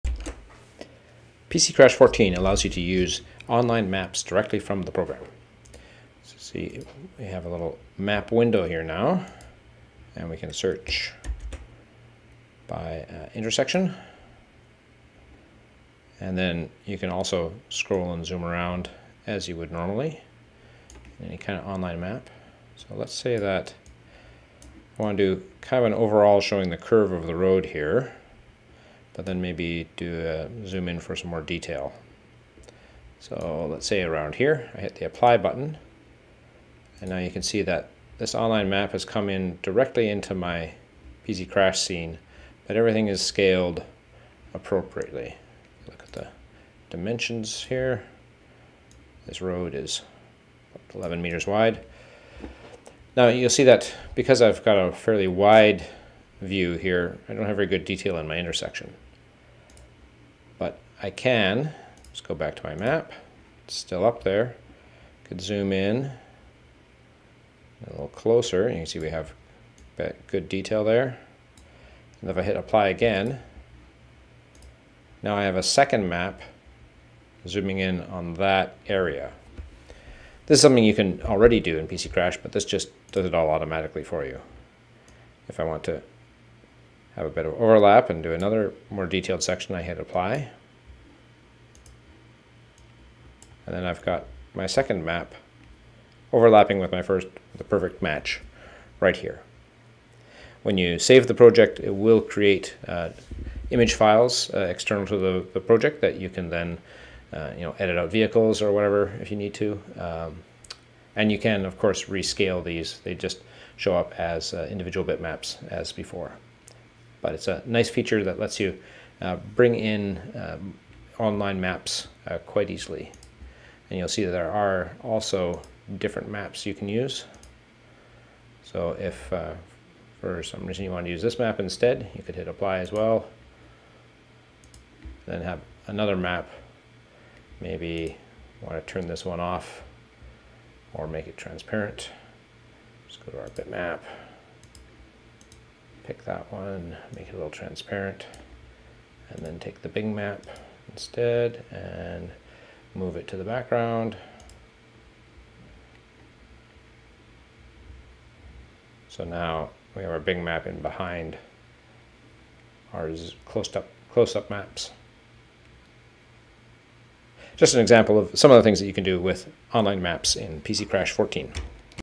PC-Crash 14.0: Online Maps
PC-Crash 14.0 has integrated online mapping.  Scene aerials are now easier to import and scale automatically.  Multiple overlapping aerial images are easy.